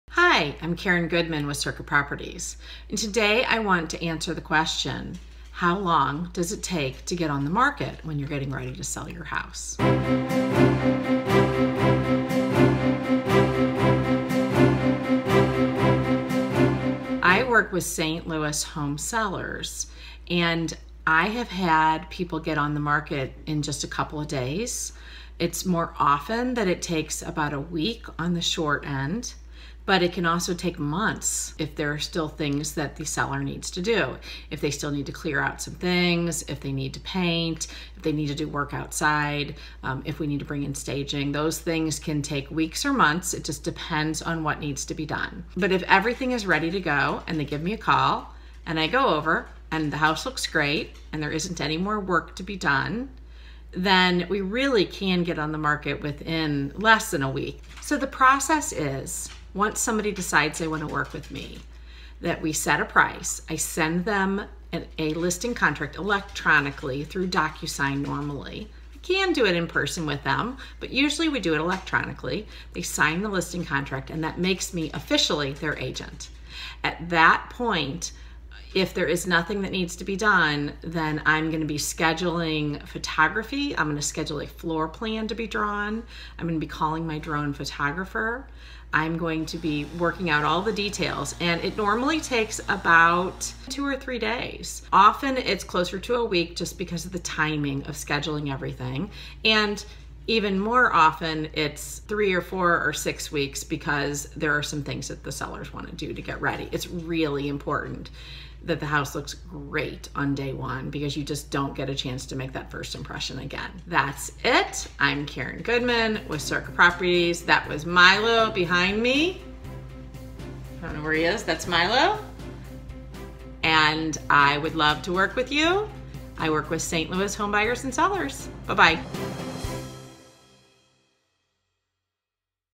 How Long Does it Take to Get on the Market to Sell
Visit Arch City Homes to see more video tips. Karen Goodman with Circa Properties in St. Louis, shares how long it takes to get a home on the market to sell. How much time? Is it days, weeks, months? What needs to be done before you go on the market? Why can't you just get on the market fast and then do those things later?